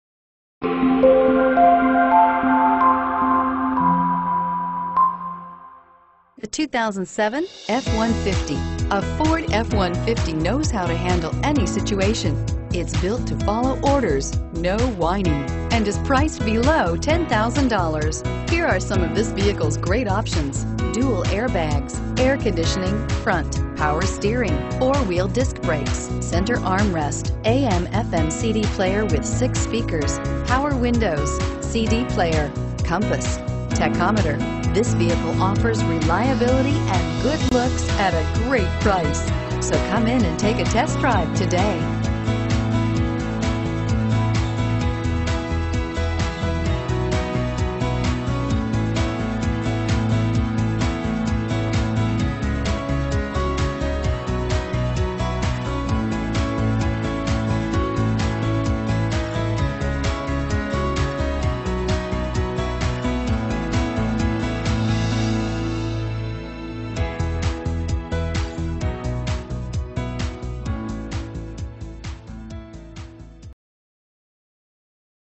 2007 Ford F-150 Montgomery AL A2219B
2007 Ford F-150 XLT

227 Eastern Blvd.

Pick up this Oxford White Clearcoat 2007 Ford F-150, available today at Jack Ingram Motors. Featuring Automatic transmission, it has 258316 miles. This could be the one you've been searching for.